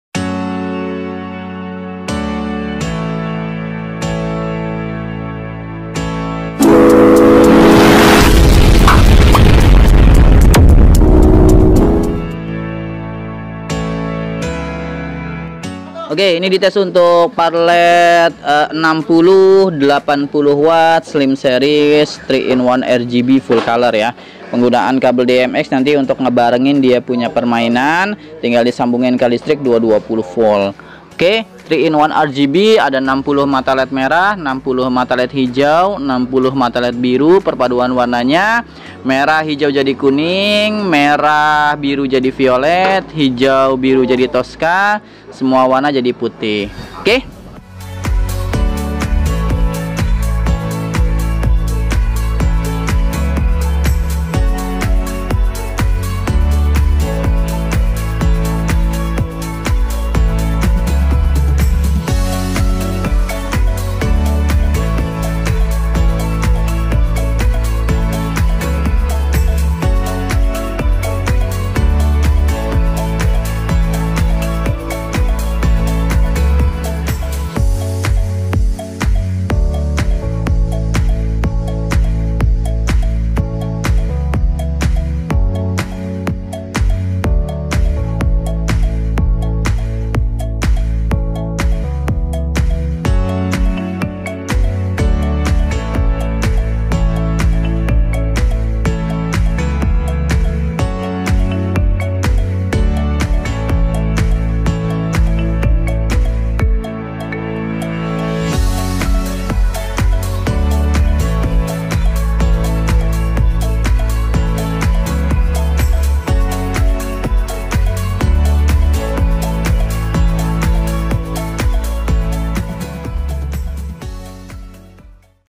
LED Par RGB Fullcolor 80W Slim Series
Seru nih pembahasan banglighting hari ini,    LED Par RGB Fullcolor 80W Slim Series - Yuk langsung aja simak hehe

Mic :
Secondary : BOYA BY-M1 Lavalier Clip On

Tripod : Takara ECO-196A